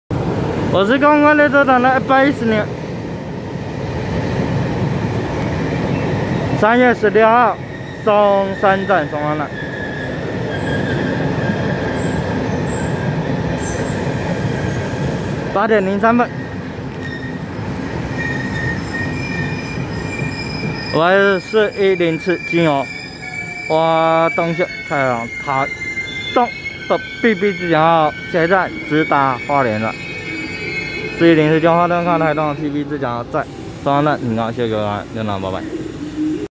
410次經由花東線開往台東的PP自強號
110/3/16松山站8:03分拍攝410次經由花東線開往台東的PP自強號